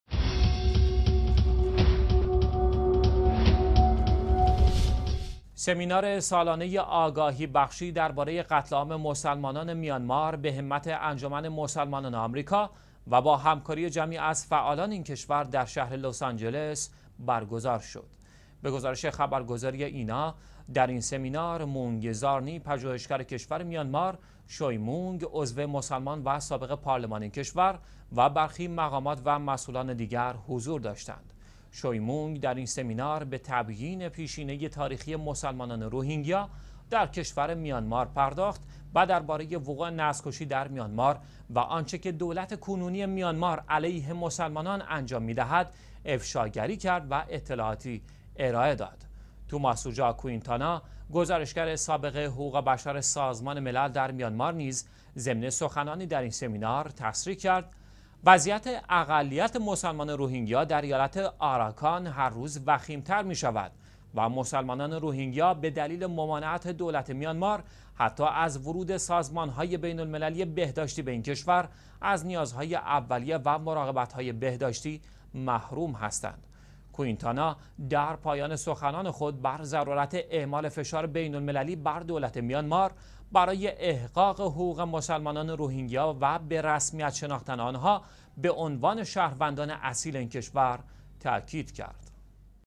برگزاری سمینار «افشاگری در باره كشتار مسلمانان میانمار» در آمریکا/ ۲۷ مرداد ۱۳۹۵
برگزاری سمینار «افشاگری در باره كشتار مسلمانان میانمار» در آمریکا

سمینار سالانه «آگاهی‌بخشی نسبت به قتل‌عام مسلمانان میانمار» به همت انجمن مسلمانان آمریکا و با همکاری جمعی از فعالان این کشور در شهر «لس‌آنجلس» برگزار شد.

به گزارش خبرگزاری اینا، در این سمینار، «مونگ زارنی»، پژوهشگر کشور میانمار، «شوی مونگ»، عضو مسلمان سابق پارلمان این کشور و برخی مقامات و مسئولان دیگر حضور داشتند.

شوی مونگ، در این سمینار به تبیین پیشینه تاریخی مسلمانان روهینگیا در کشور میانمار پرداخت و اطلاعاتی ارائه داد که بر وقوع نسل_کشی در میانمار و آنچه که دولت کنونی میانمار علیه مسلمانان انجام می‌دهد، تاکید می‌کند.

«توماس اوجا کوینتانا»، گزارشگر سابق حقوق_بشر سازمان ملل در میانمار نیز ضمن سخنانی در این سمینار تصریح کرد: وضعیت اقلیت مسلمان روهینگیا در ایالت «آراکان» هر روز وخیم‌تر می‌شود و مسلمانان روهینگیا به دلیل ممانعت دولت میانمار حتی از ورود سازمان‌های بین‌المللی بهداشتی به این کشور از نیازهای اولیه و مراقبت‌های بهداشتی محروم هستند.

کوینتانا در پایان سخنان خود بر ضرورت اعمال فشار بین المللی بر دولت میانمار برای احقاق حقوق مسلمانان روهینگیا و به رسمیت شناختن آن‌ها به عنوان شهروندان اصیل این کشور تأکید کرد.